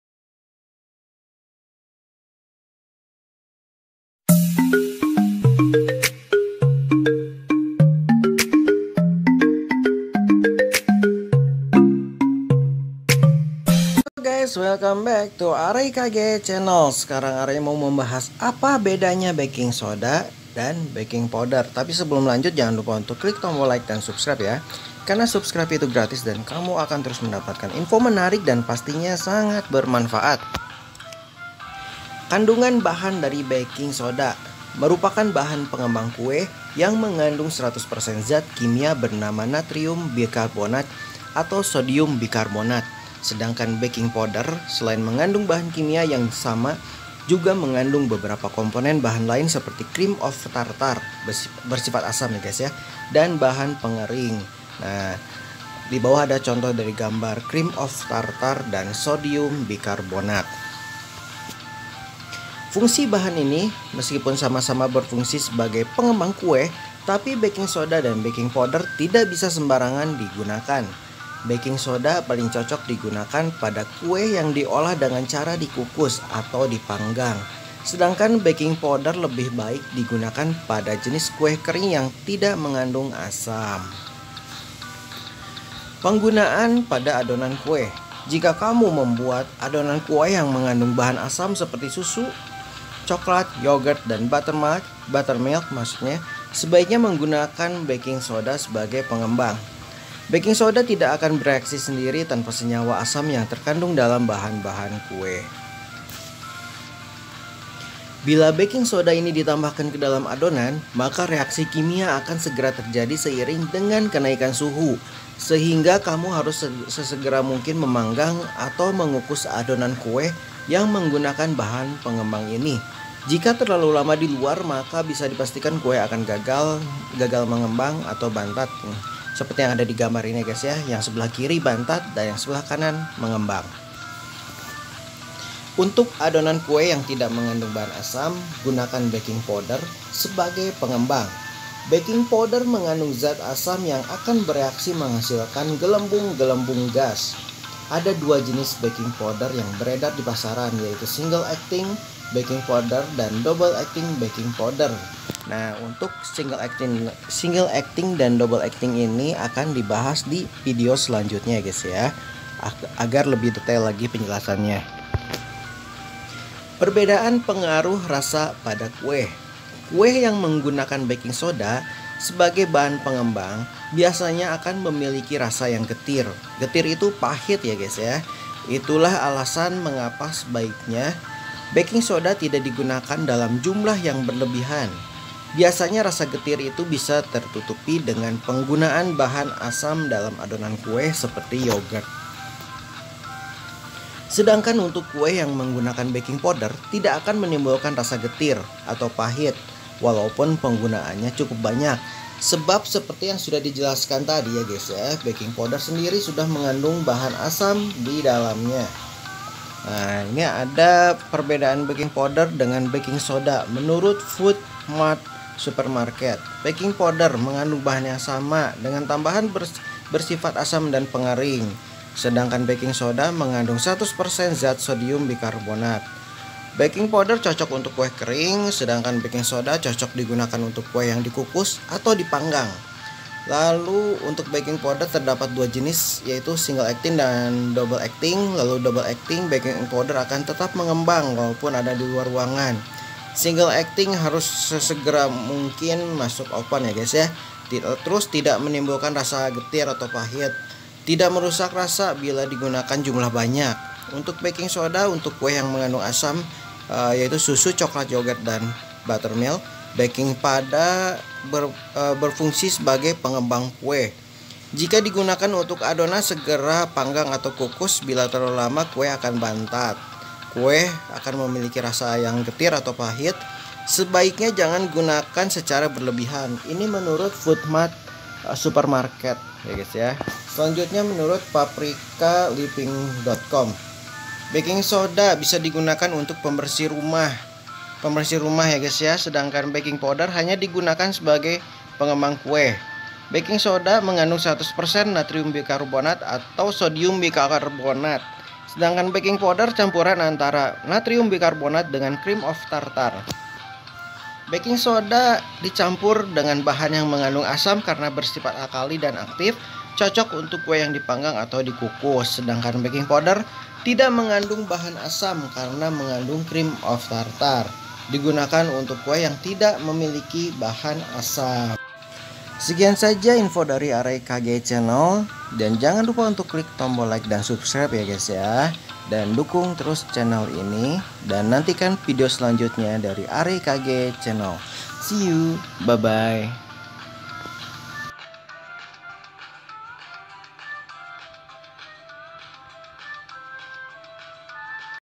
Perbedaan baking soda dan baking powder
apa sajakah perbedaan keduanya ini, mari disimak